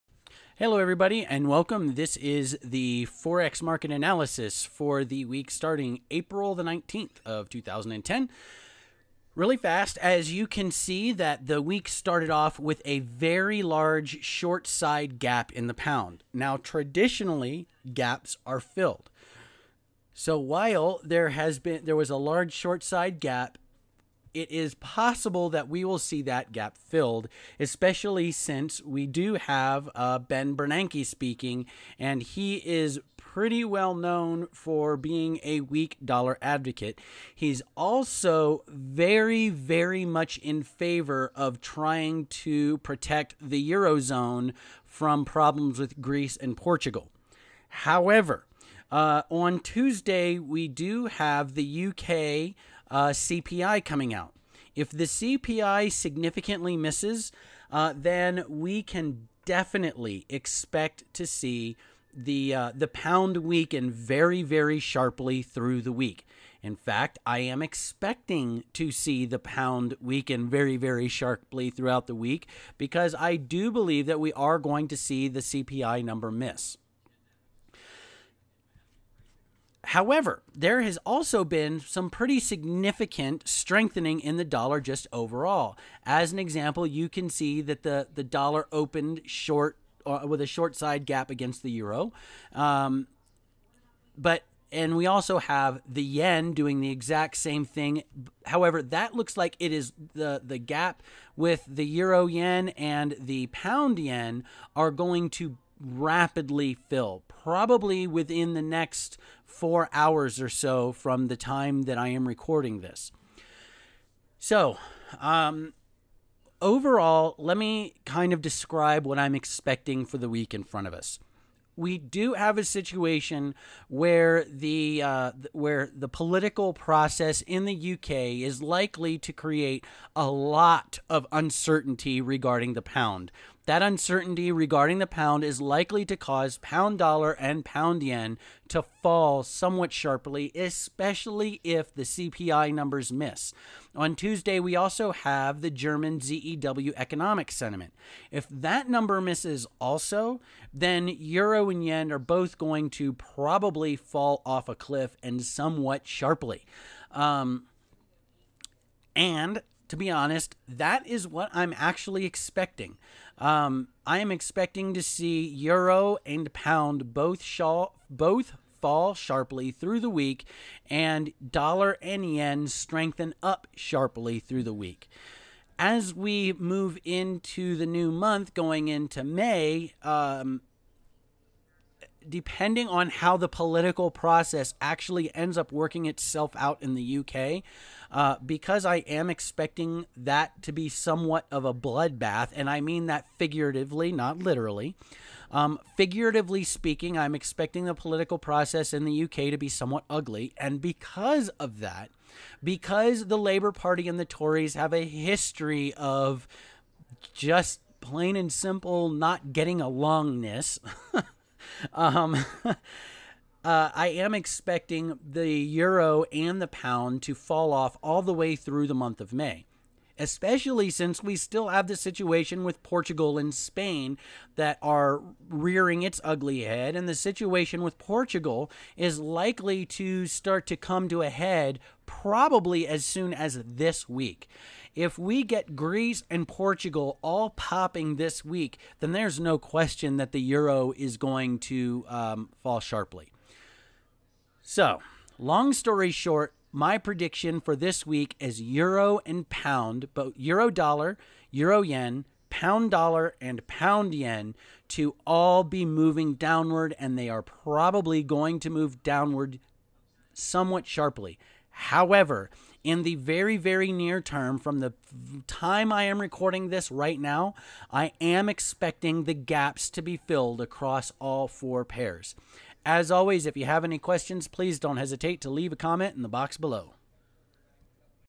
Forex Market Analysis 4/19/2010
Forex market analysis covering the dollar, pound, euro, and yen